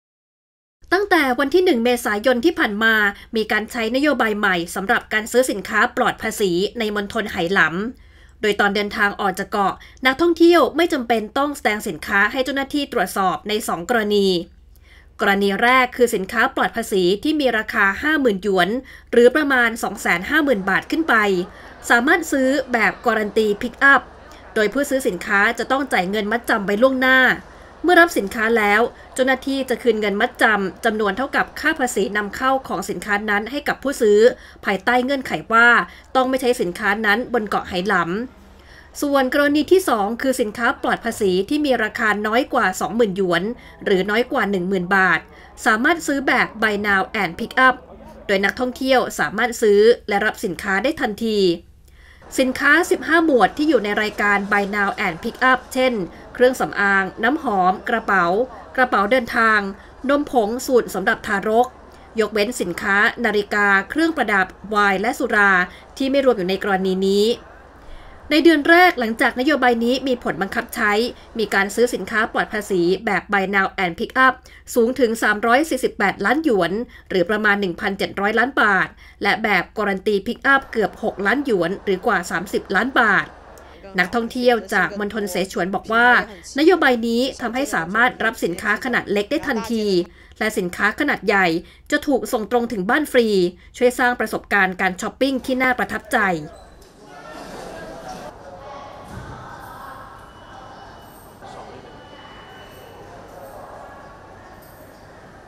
นโยบายปลอดภาษีใหม่ในไหหลำสร้างประสบการณ์ใหม่ให้นักช็อป (พากย์ไทย)
ตั้งแต่วันที่ 1 เมษายนที่ผ่านมา มีการใช้นโยบายใหม่สำหรับการซื้อสินค้าปลอดภาษีในมณฑลไหหลํา โดยตอนเดินทางออกจากเกาะ นักท่องเที่ยวไม่จำเป็นต้องแสดงสินค้าให้เจ้าหน้าที่ตรวจสอบใน 2 กรณี
กรณีแรก คือ สินค้าปลอดภาษีที่มีราคา 50,000 หยวน หรือ 250,000 บาทขึ้นไป สามารถซื้อแบบ "guaranteed pick-up" โดยผู้ซื้อสินค้าจะต้องจ่ายเงินมัดจำไว้ล่วงหน้า เมื่อรับสินค้าแล้ว เจ้าหน้าที่จะคืนเงินมัดจำจำนวนเท่ากับค่าภาษีนำเข้าของสินค้านั้นให้กับผู้ซื้อ ภายใต้เงื่อนไขว่าต้องไม่ใช้สินค้านั้นบนเกาะไหหลำ
กรณีที่สอง คือ สินค้าปลอดภาษีที่มีราคาน้อยกว่า 20,000 หยวน หรือน้อยกว่า 10,000 บาท สามารถซื้อแบบ “buy now and pick-up” โดยนักท่องเที่ยวสามารถซื้อและรับสินค้าได้ทันที
สินค้า 15 หมวด ที่อยู่ในรายการ “buy now and pick-up” เช่น เครื่องสําอาง น้ำหอม กระเป๋า กระเป๋าเดินทาง นมผงสูตรสําหรับทารก ยกเว้นสินค้า นาฬิกา เครื่องประดับ ไวน์ และสุรา ไม่รวมอยู่ในกรณีนี้
ในเดือนแรก หลังจากนโยบายฯ นี้มีผลบังคับใช้ มีการซื้อสินค้าปลอดภาษีแบบ “buy now and pick-up” สูงถึง 348 ล้านหยวน หรือประมาณ 1,700 ล้านบาท และแบบ "guaranteed pick-up" เกือบ 6 ล้านหยวน หรือกว่า 30 ล้านบาท
Huang Jingjing นักท่องเที่ยวจากมณฑลเสฉวนบอกว่า นโยบายนี้ทำให้สามารถรับสินค้าขนาดเล็กได้ทันทีและสินค้าขนาดใหญ่จะถูกส่งตรงถึงบ้านฟรี ช่วยสร้างประสบการณ์การช้อปปิ้งที่น่าประทับใจ